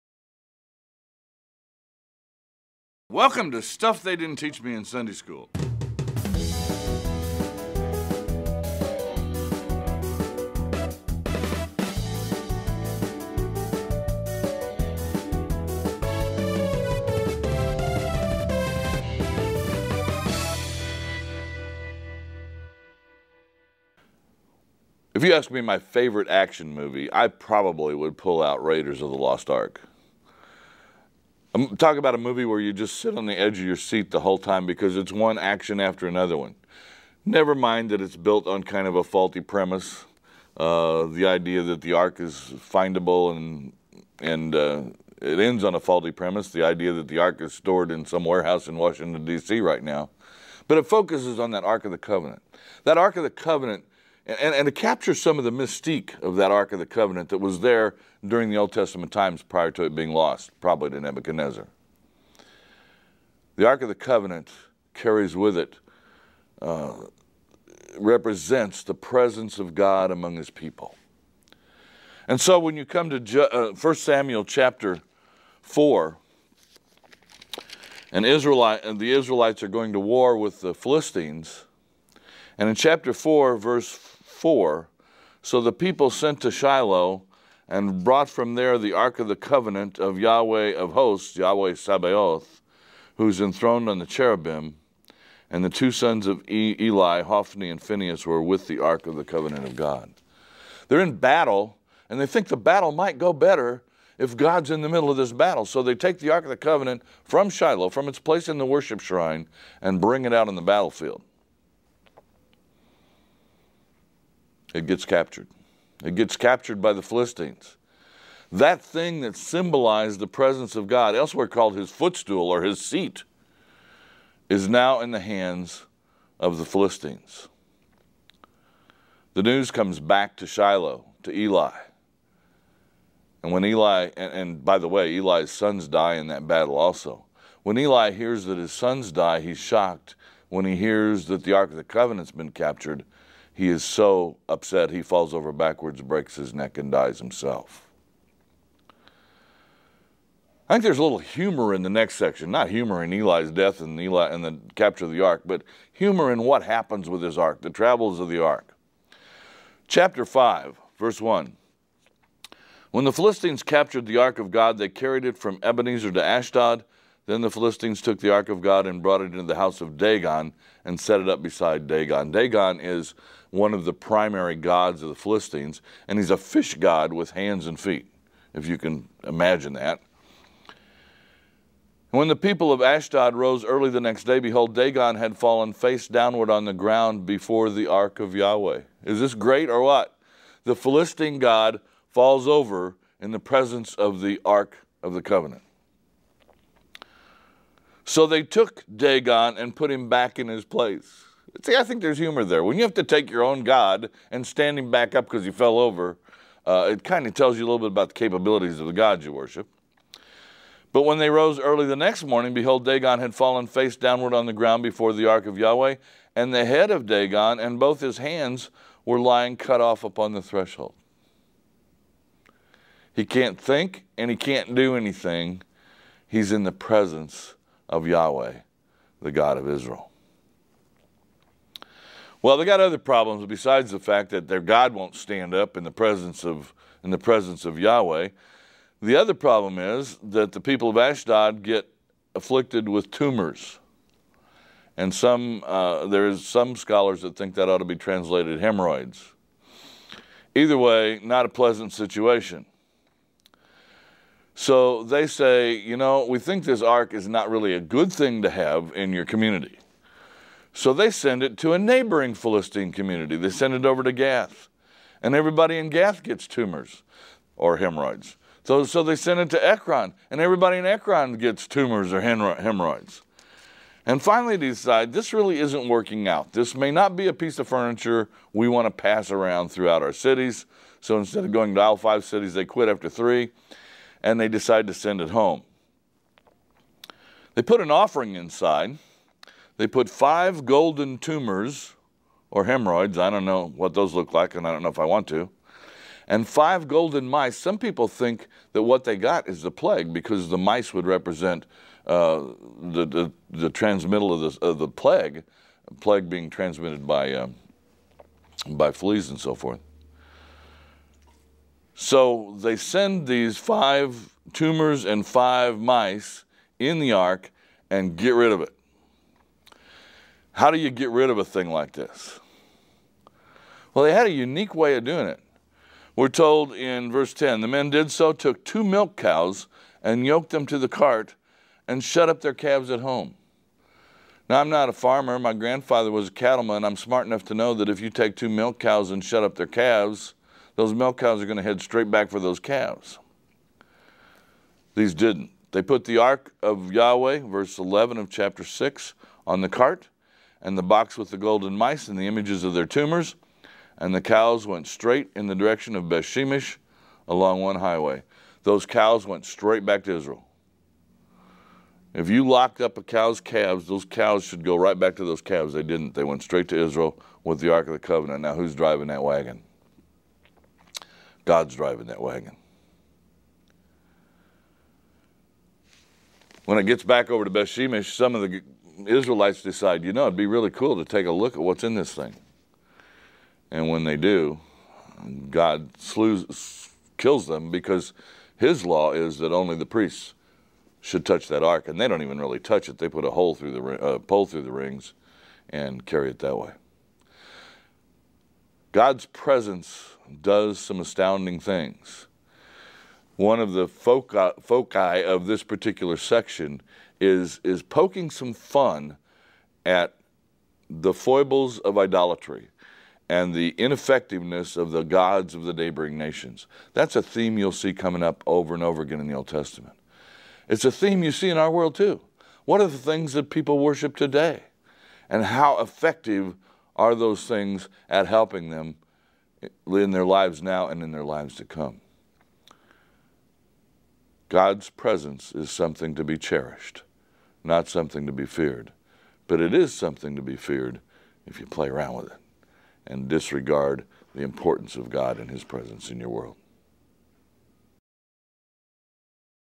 What Were They Thinking?
Israel's enemies capture God's ark and hold it hostage in the temple of their god. Big mistake!